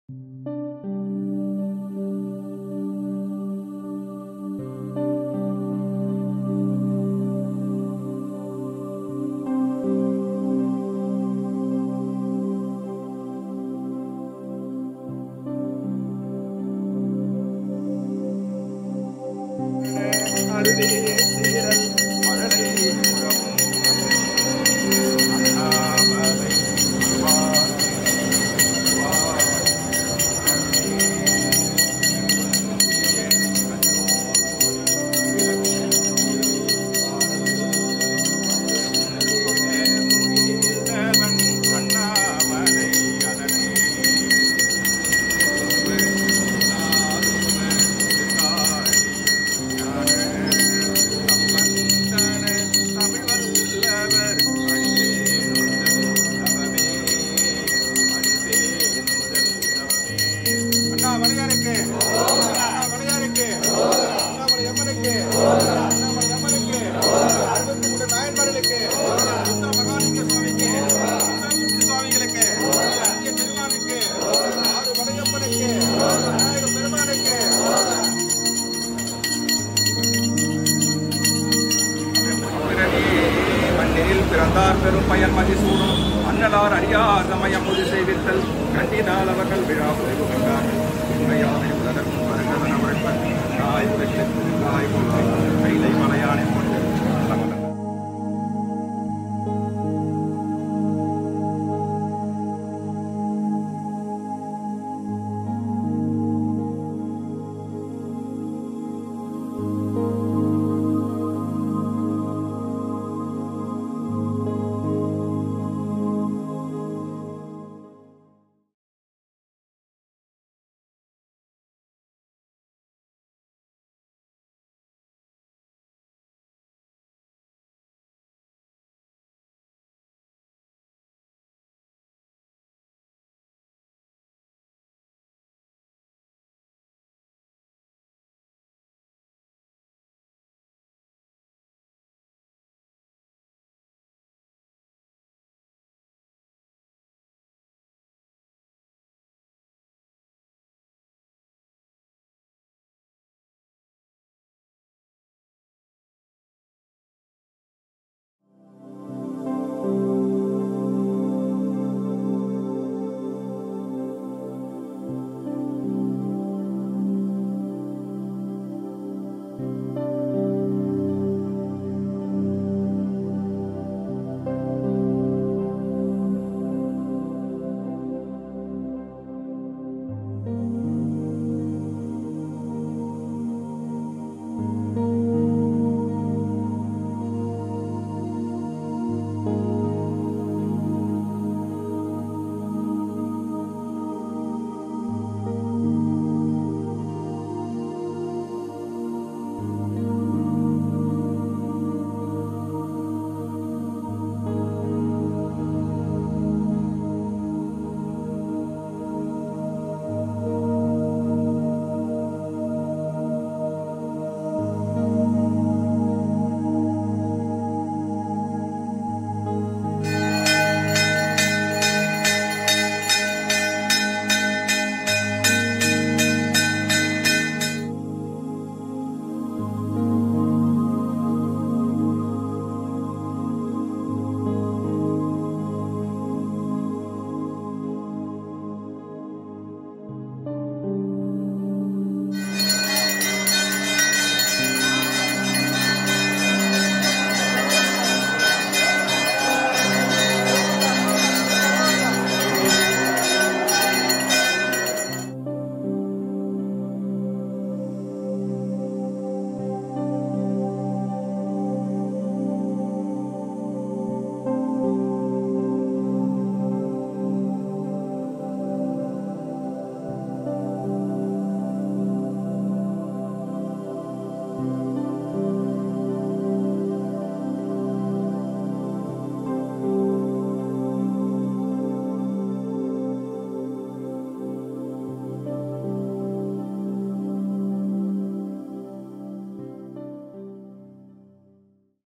Karthigai Deepam Maha Anndhanam 2023 @NayanarsGuruseva Tiruvannamalai
The grand Karthigai Deepam festival is held annually in the month of Karthigai 20Lakh Pilgrims, monks, and sadhus come to have darshan on this Deepam festival. Perform Annadhana Sevas on this Auspicious day and gain Blessings.